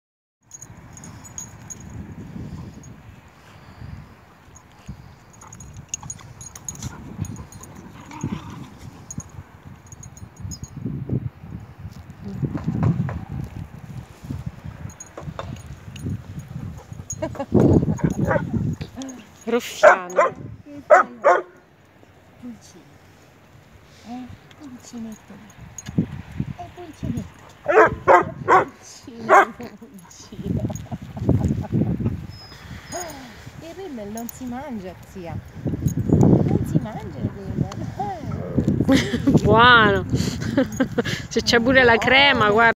Cani Taglia Media maschi in regalo
Coinvolgente, divertente, affettuoso come un turbine d'amore. E bello, molto bello. Blues ha un anno e mezzo, va al guinzaglio, in macchina, è sterilizzato e socializzato con maschi e femmine. Che gli manca? Una casa. E' affettuoso tanto da spalmartisi addosso. non vorrebbe lasciarti più. Pesa appena 12 kg...è il cane che chiunque vorrebbe avere...eppure...è ancora senza una famiglia. Cambiamo le note della sua vita?